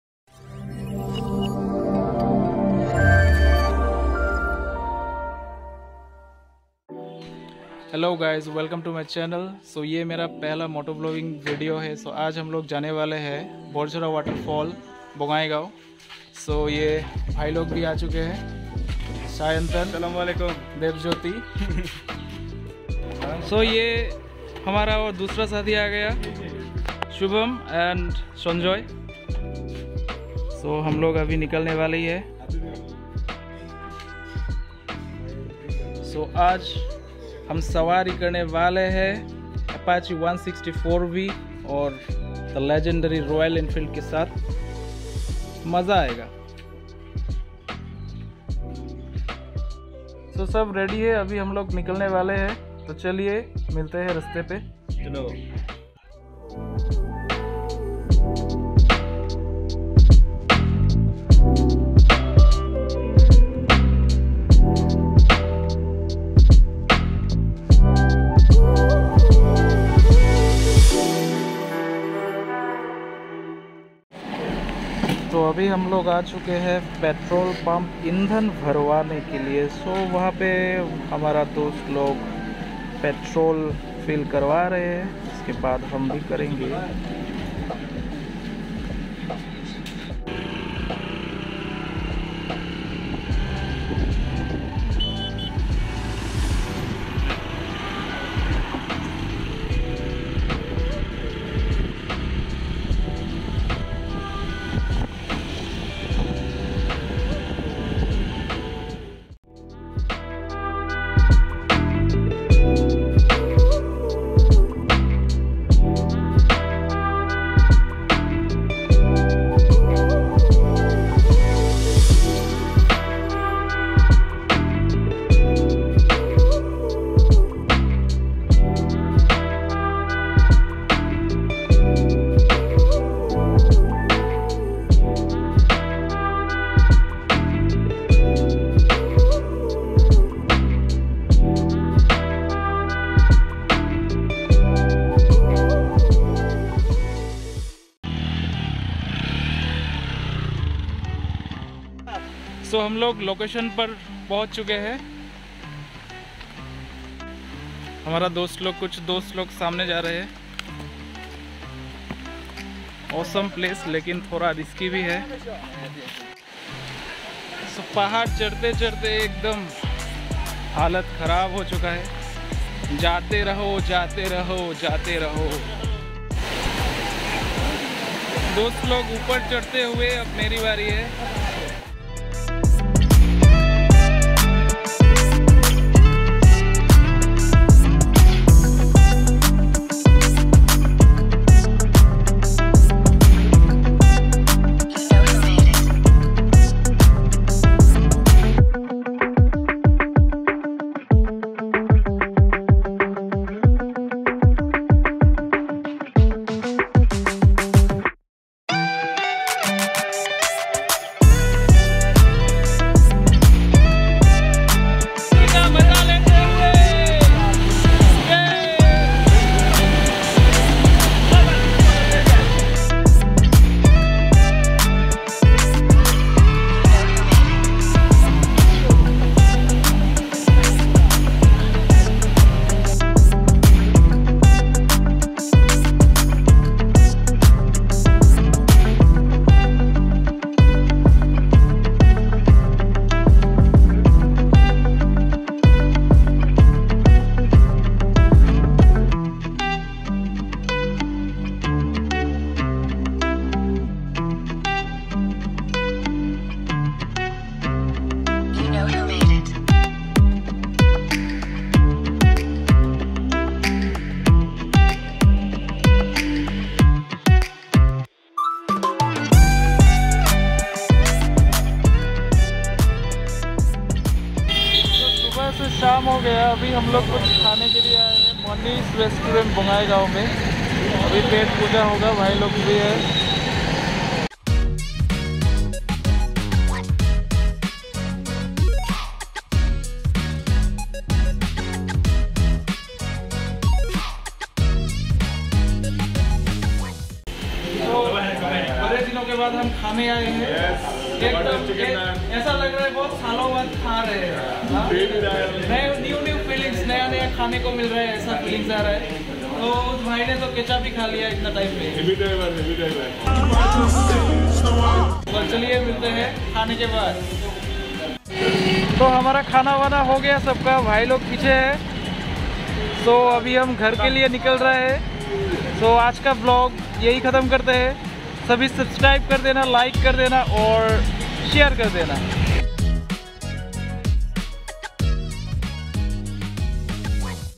BORJHORA WATER FALL || A Cinematic Moto Vlog
Hi Guys ! This is my 1st moto blog Video. More Videos Coming Soon...